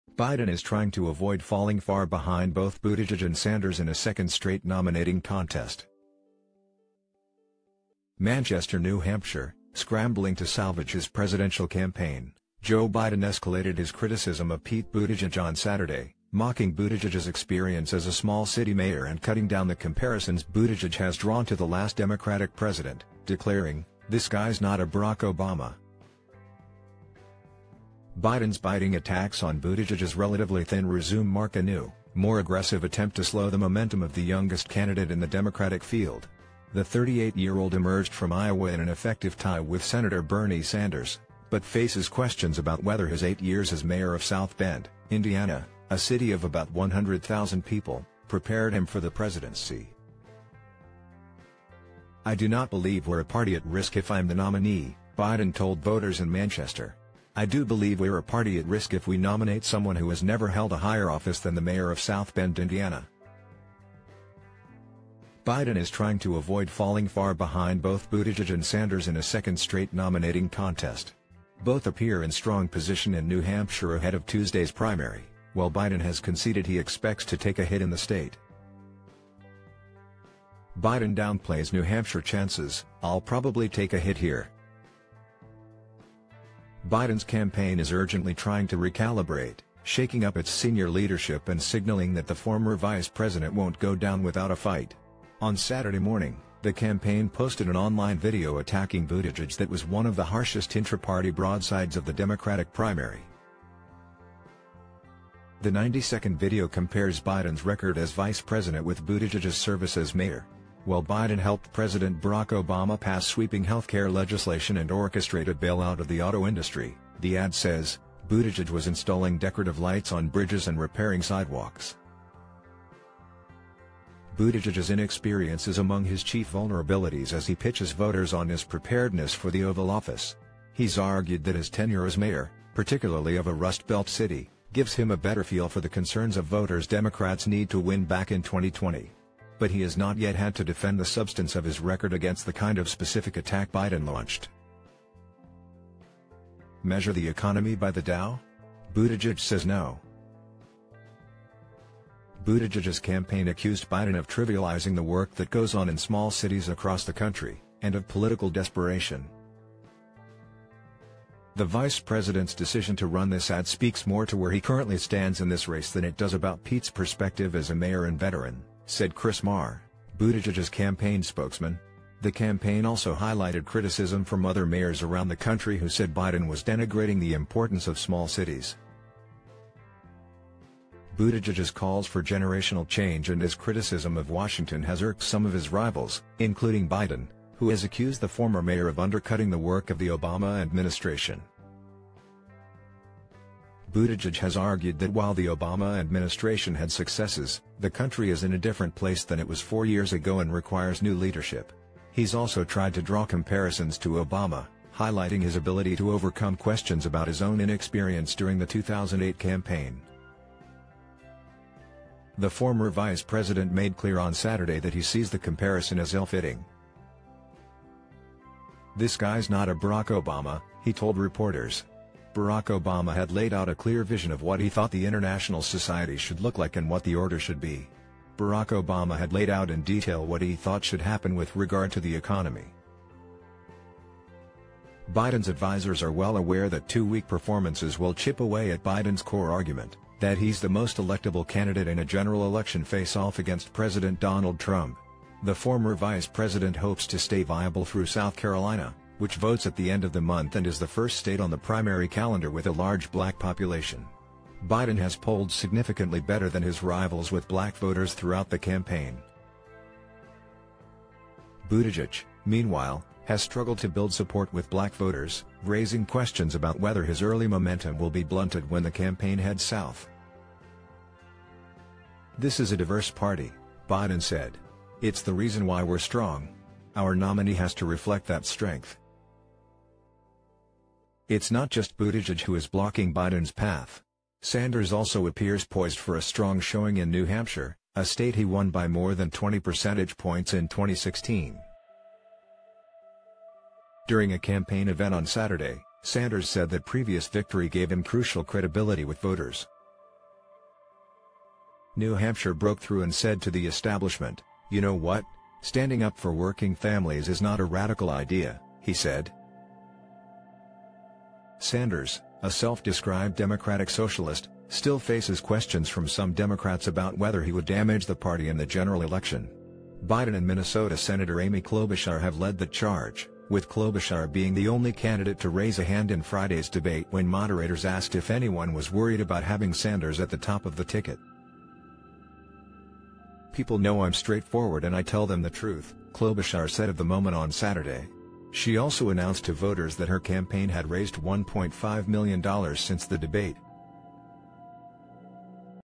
Biden tells New Hampshire Democrats that Buttigieg ‘not a Barack Obama’
Biden is trying to avoid falling far behind both Buttigieg and Sanders in a second straight nominating contest.